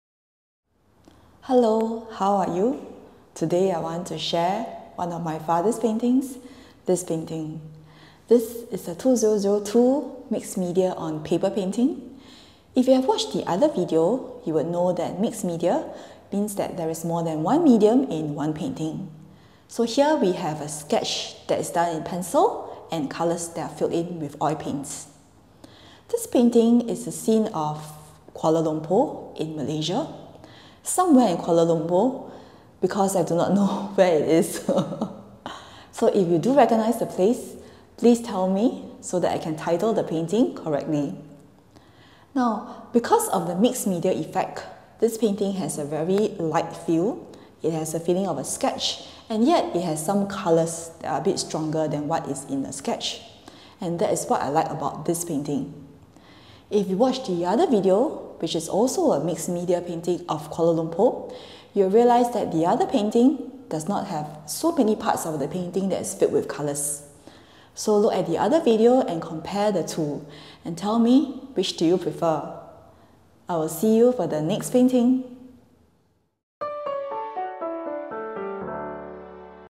My Father's Paintings No 66 (painting of Kuala Lumpur by Low Hai Hong)
Painting No 66 is a mixed media painting (pencil sketch with oil colours) of Kuala Lumpur, Malaysia.  Need help! Anyone know where is this place?

This painting is available for collection.

My father is the Singapore artist Low Hai Hong.  He passed away in 2020 and left me his paintings.  I want to share his lifetime of work with the world and I hope that his paintings will bring you joy and peace.